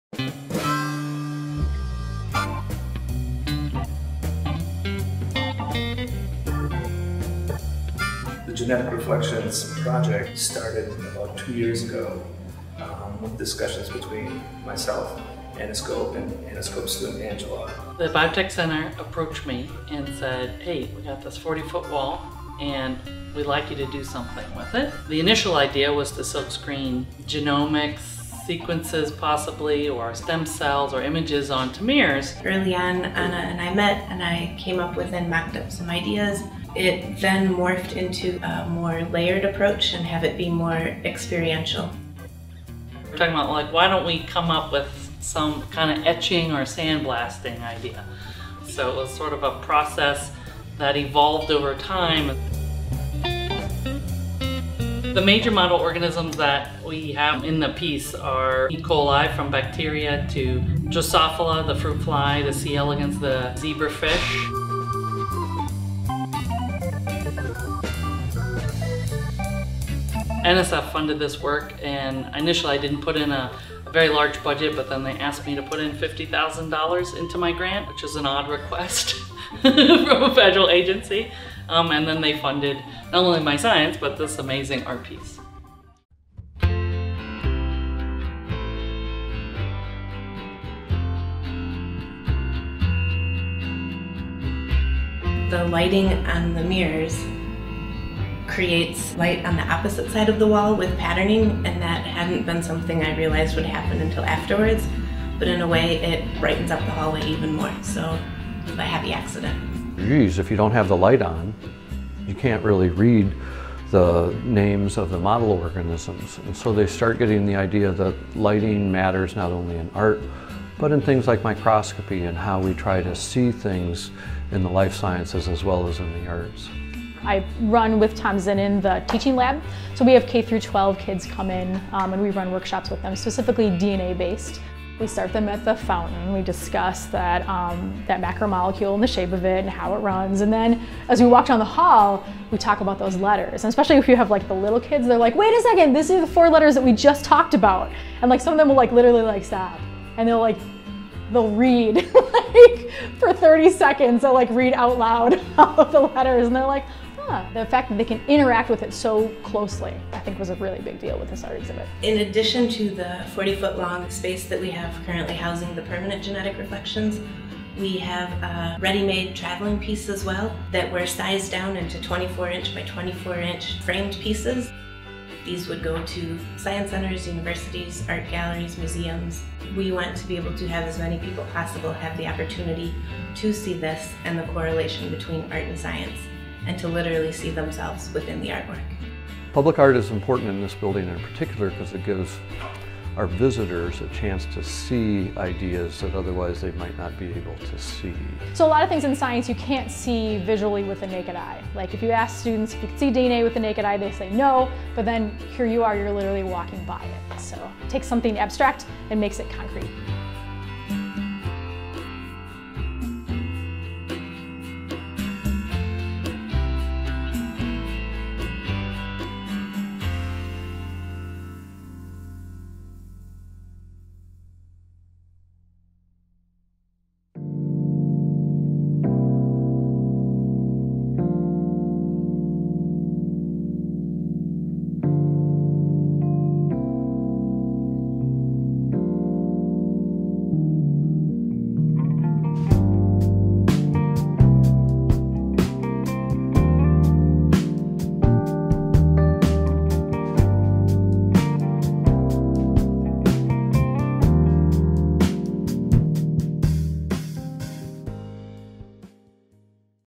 Genetic Reflections
“Genetic Reflections,” is a public science art installation created by Madison-based artist Angela Johnson in collaboration with UW–Madison genetics professor Ahna Skop. The piece is permanently installed outside of the Genetics/Biotechnology Center auditorium across from Room 1360.

The science art piece is designed to highlight genetic research on campus and encourage visitors to interact and support science.

Funding and supplies for the installation were provided by the National Science Foundation, the UW–Madison Biotechnology Center, and the Wisconsin Materials Research Science and Engineering Center.